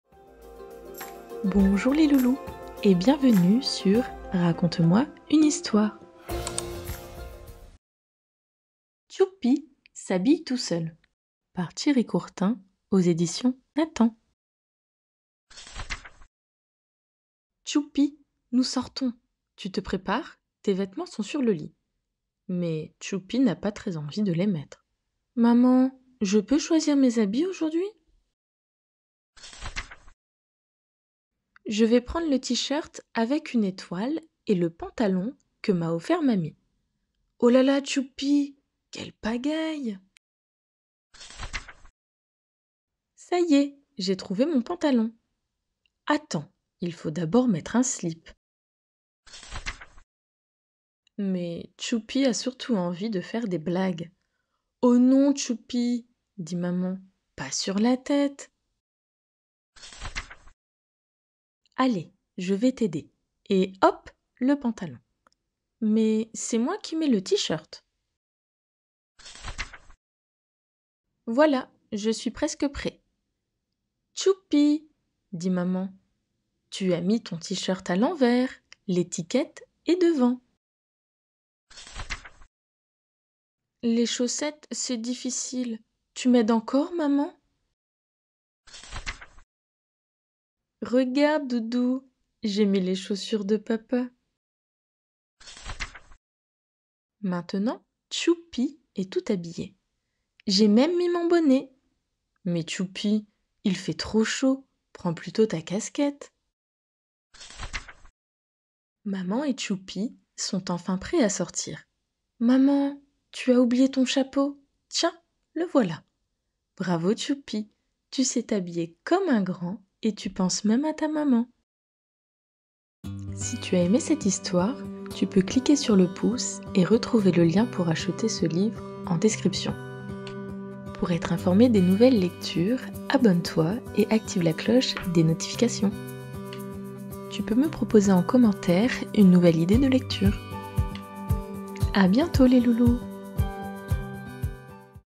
Lecture : Tchoupi s'habille tout seul
Lecture de livre pour bébés & enfants

Titre : T'choupi s'habille tout seul
Édition : Nathan
Auteur : Thierry Courtin

Résumé : Aujourd'hui, T'choupi a décidé de choisir ses vêtements, comme un grand !